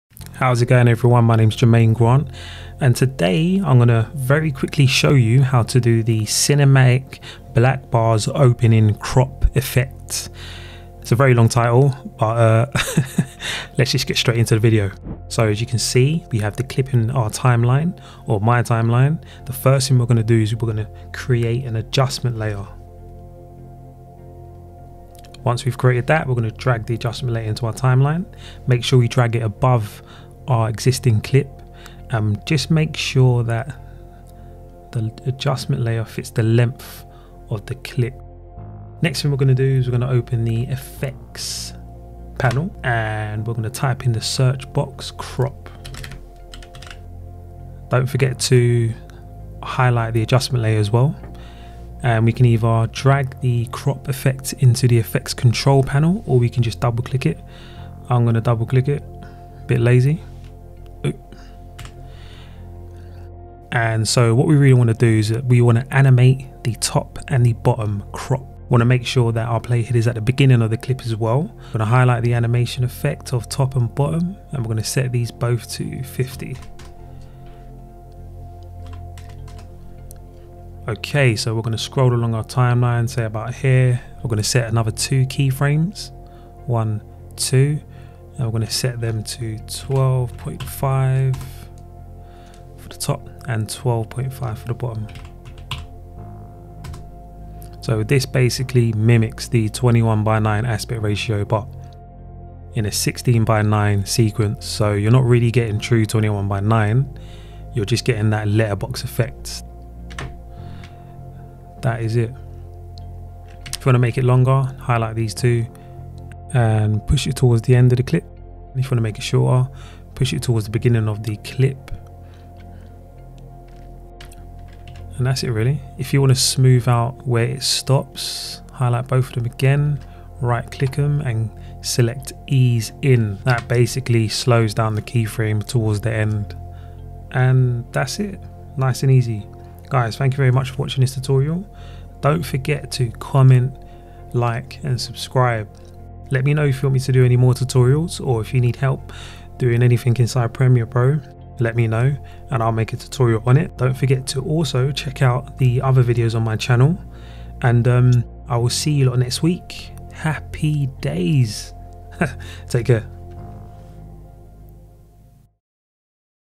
Adobe Premiere Pro CC Tutorial: Cinematic Animated Black Bars Open Effect
Easy Quick Tutorial on how to do the Animated Black Bars opening effect in Premiere Pro for all that Cinematic Wonderfulness!!